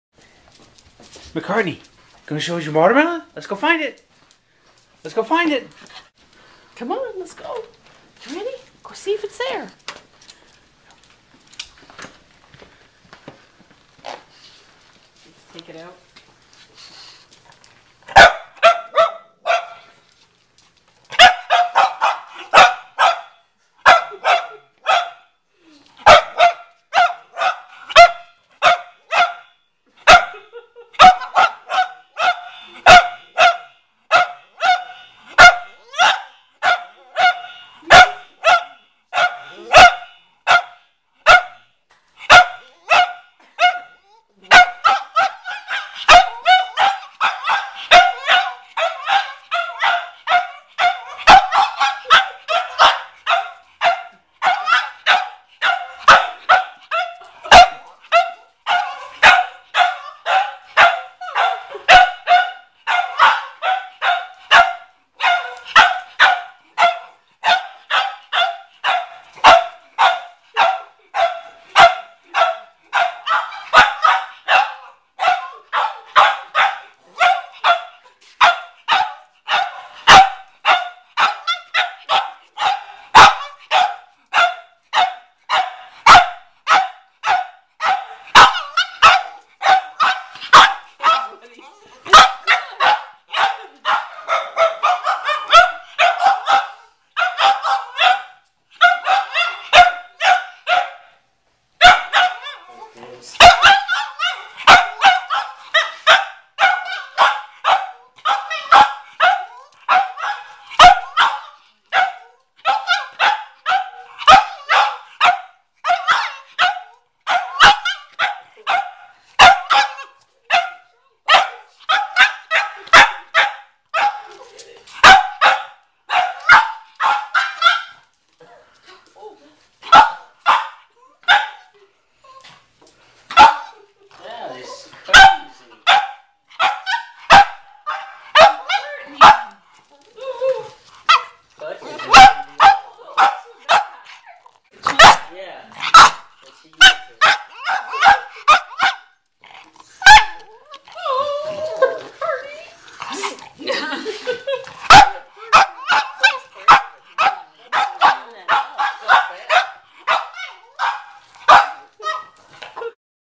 Bad Bad Watermelon.m4v
Our Cavalier King Charles Spaniel, McCartney, saw a watermelon in our garage one day and he proceeded to bark at it. Brought him inside, and each time he went to the garage, he would do the same thing. Our English Bulldog, on the other hand could have cared less!